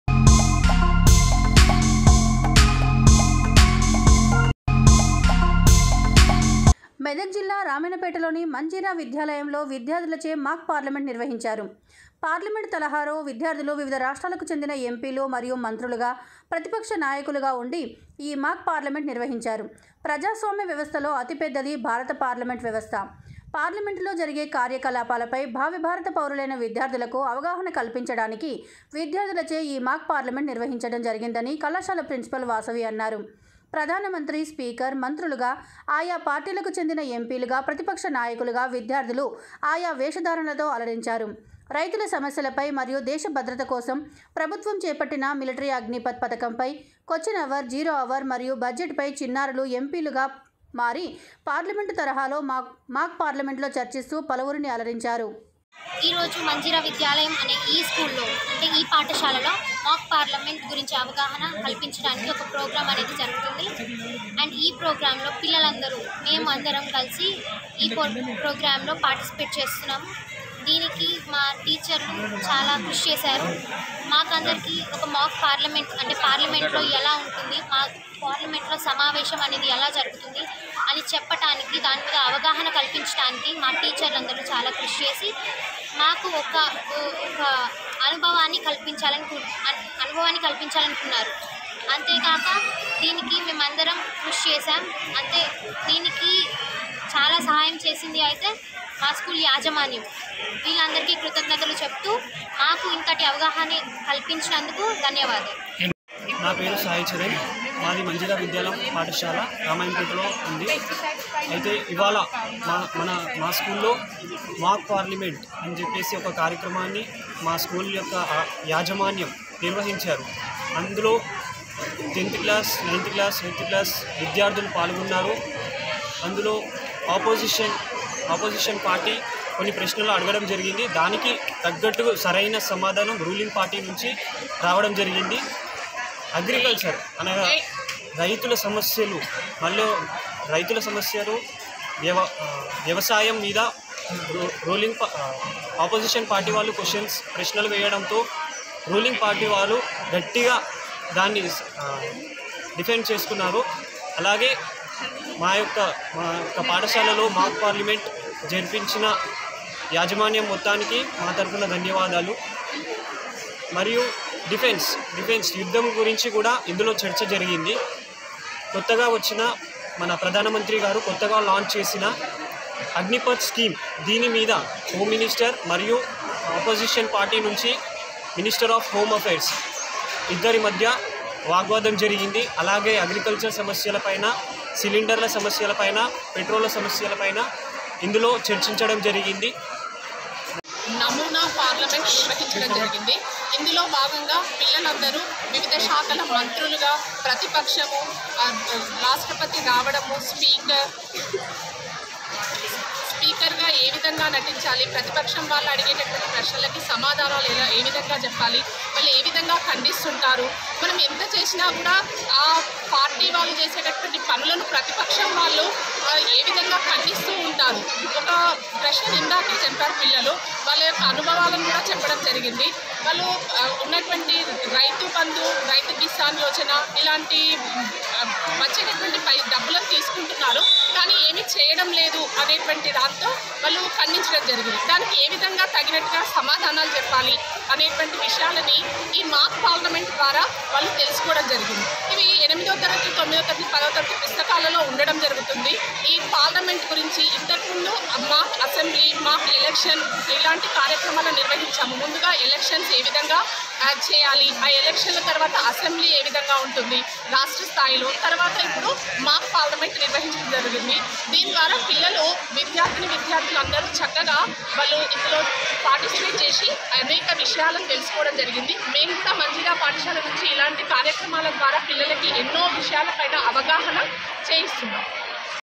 రామాయంపేటలోని మంజీరా విద్యాలయంలో విద్యార్థులచే మాక్ పార్లమెంటు నిర్వహించారు.
మెదక్ జిల్లా రామాయంపేటలోని మంజీరా విద్యాలయంలో విద్యార్థులచే మాక్ పార్లమెంటు నిర్వహించారు. పార్లమెంటు తరహాలో విద్యార్థులు వివిధ రాష్ట్రాలకు చెందిన ఎంపీలు మరియు మంత్రులుగా ప్రతిపక్ష నాయకులుగా ఉండి ఈ మాక్ పార్లమెంట్ నిర్వహించారు.